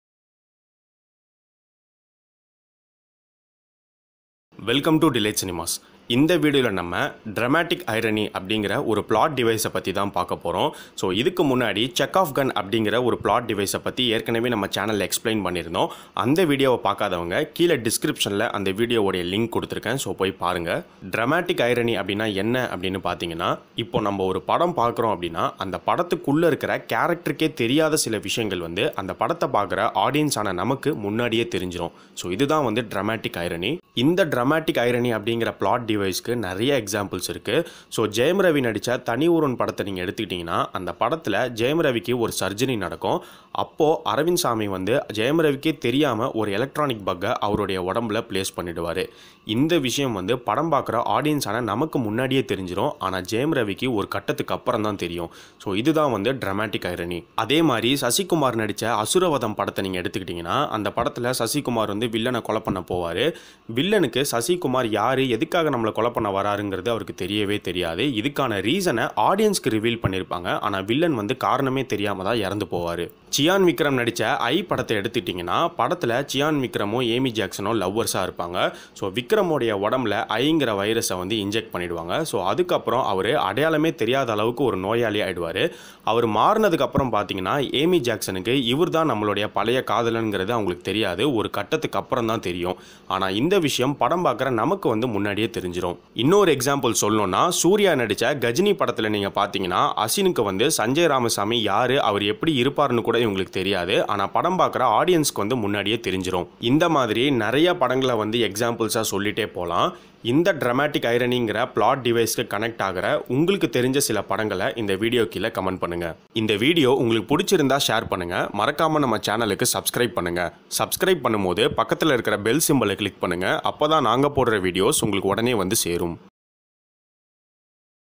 Dramatic Irony l Plot Device l Explained in Tamil l By Delite Cinemas
In this Video we are going to see "Dramatic Irony" Plot Device.
Dramatic Irony is a Plot Device Used in many movies.Watch this video fully to know more .